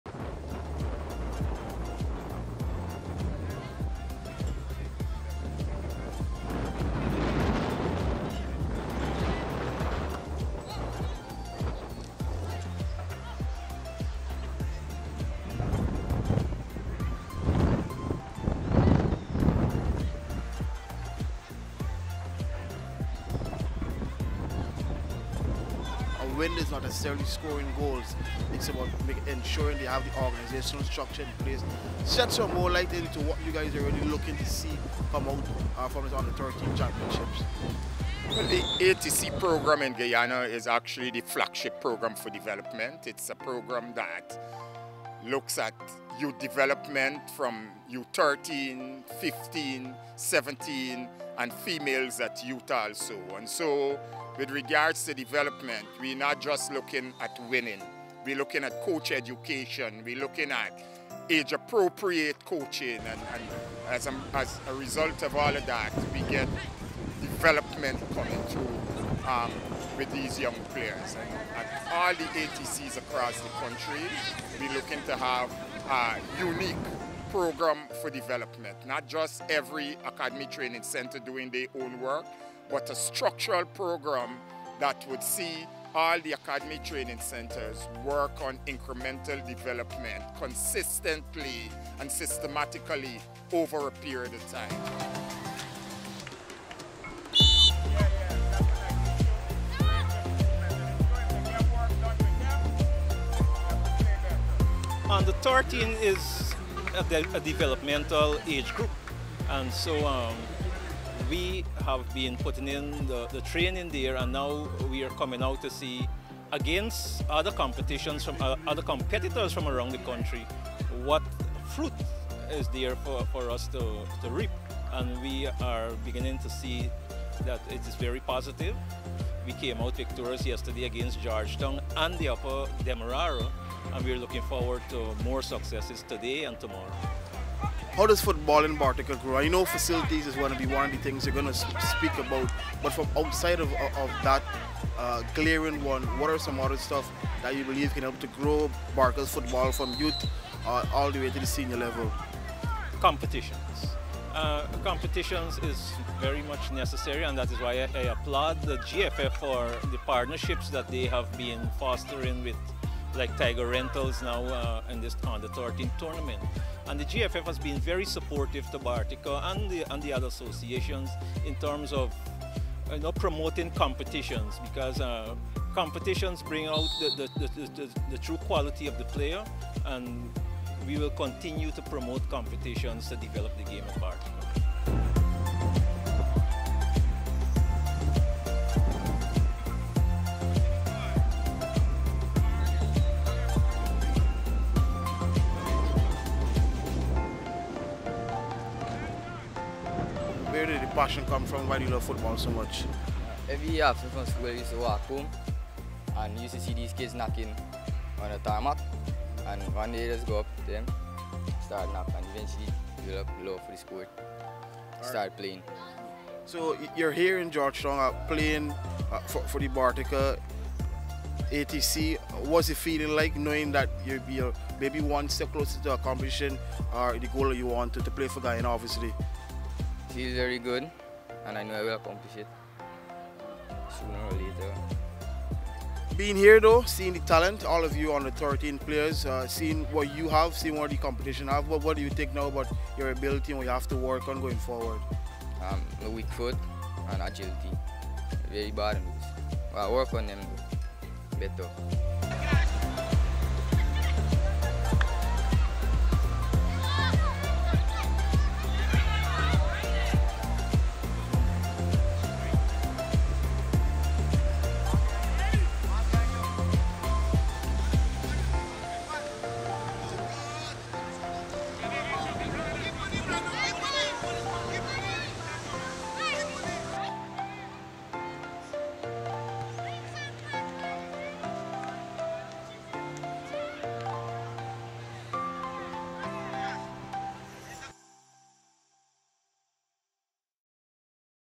‘MORE THAN WINNING’ – U 13 FOOTBALL LEAGUE AIMS FOR HOLISTIC PLAYER DEVELOPMENT
On Saturday, the Tiger Rentals Under-13 League Cup, coordinated by the Guyana Football Federation, entered its penultimate day at the GFF’s National Training Facility, Providence, and it presented another opportunity for the next generation of footballers to display their talent.
News Room’s Akeem Greene visited Providence and spoke to players, coaches, and administrators, who had a consensus that this developmental phase is the first stage of a series of building blocks to shaping future stars to represent Guyana.